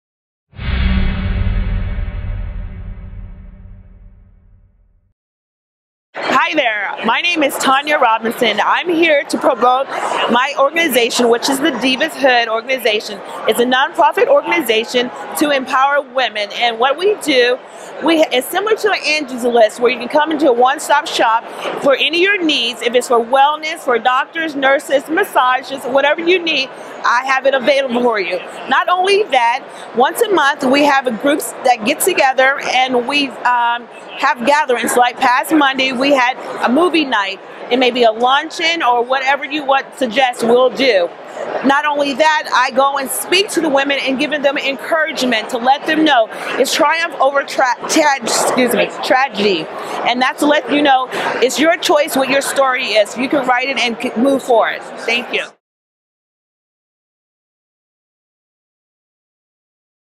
Tonia Robinson - Womens Empowerment Group
Video Business Card shot at networkingphoenix.com on Monday February 24, 2014 for Tonia Robinson - Womens Empowerment Group